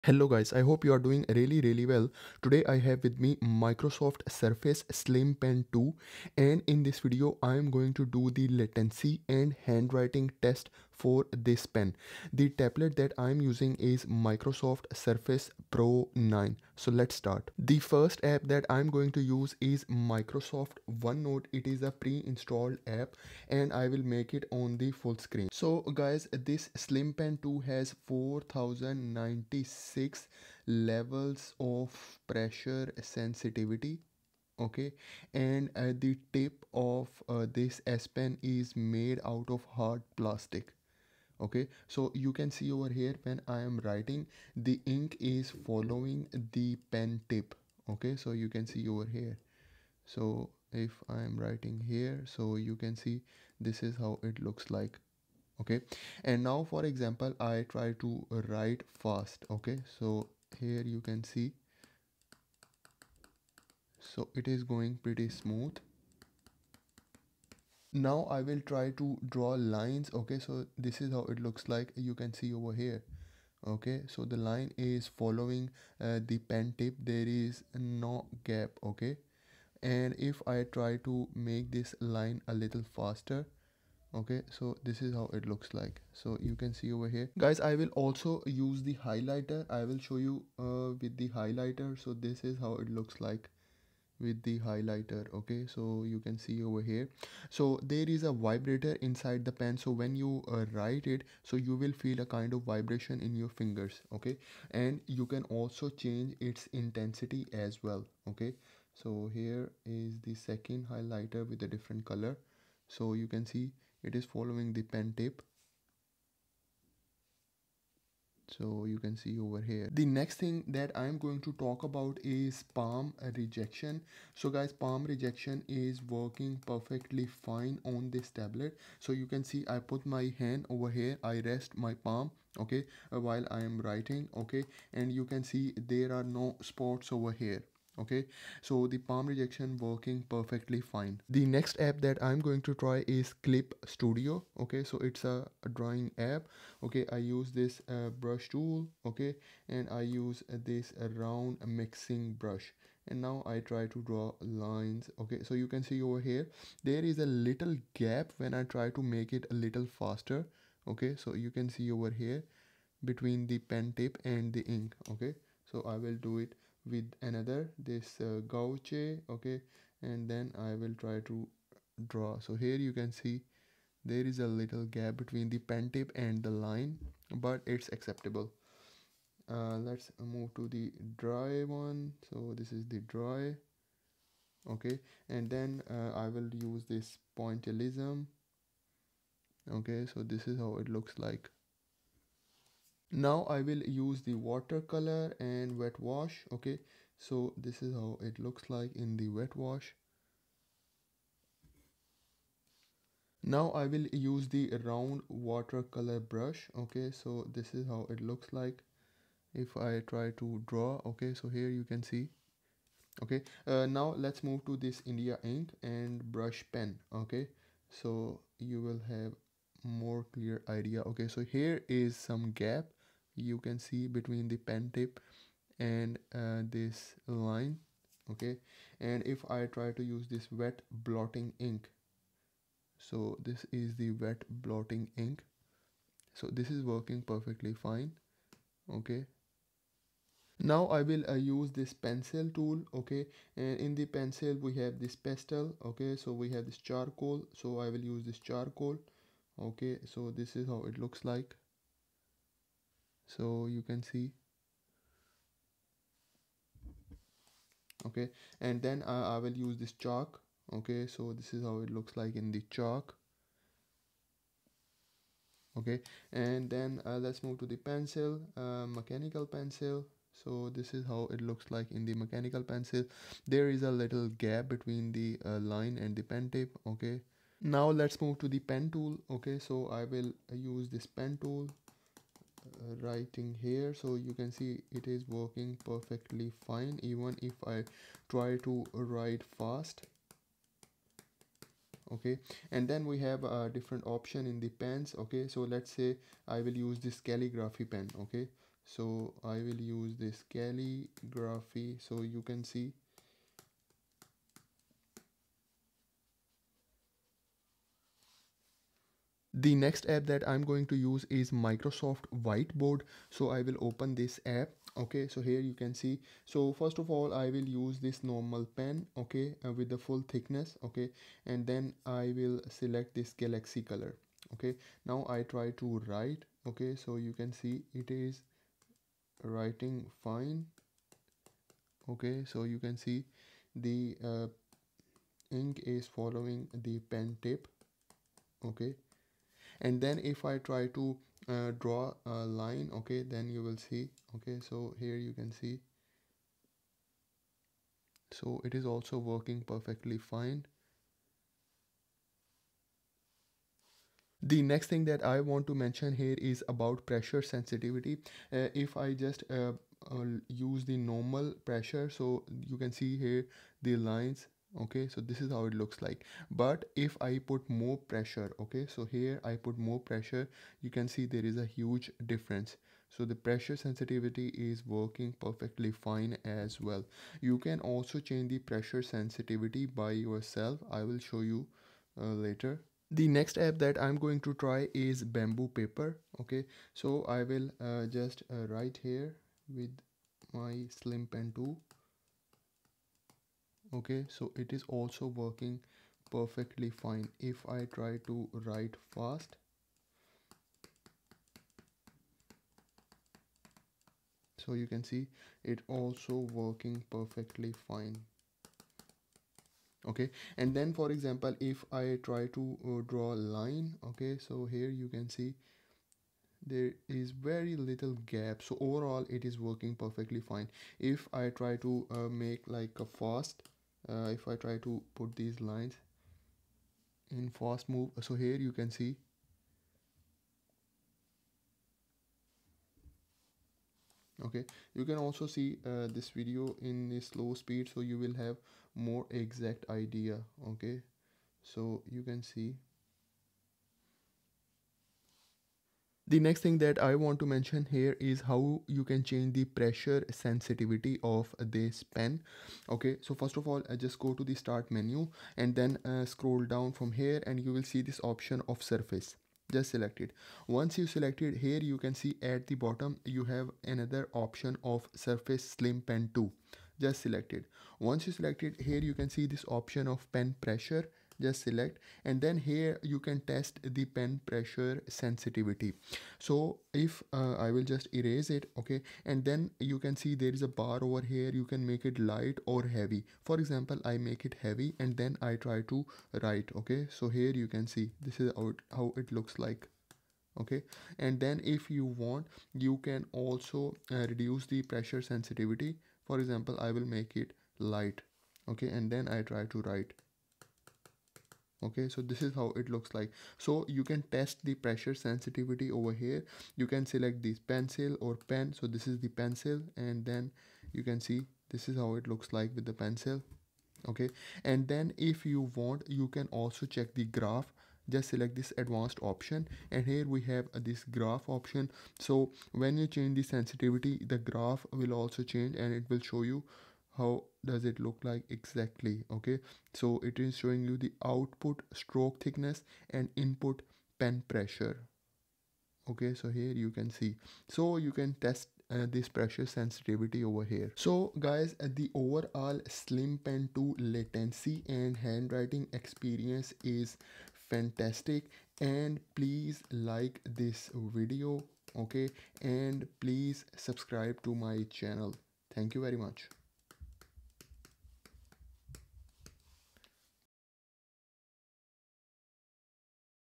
Microsoft Surface Slim Pen 2 - Latency and Handwriting Test on Surface Pro 9
Please use below links to buy Microsoft Products
Slim Pen 2
Slim Pen 2 Charging Cradle with Cable
Microsoft Surface Pro 9
Surface Pro Signature Keyboard
Microsoft Surface Products
In this video i am showing the latency and handwriting experience in for slim pen 2. The Tablet i am using is Microsoft Surface pro 9. I am using 4 different Apps. I am also showing palm rejection test and pressure sensitivity for Microsoft Surface Slim Pen 2.
Time Stamps:
00:00 - Intro
00:17 - Microsoft OneNote
2:21 - Clip Studio
5:09 - Microsoft Whiteboard
6:23 - Bamboo Paper
7:26 - Pressure Sensitivity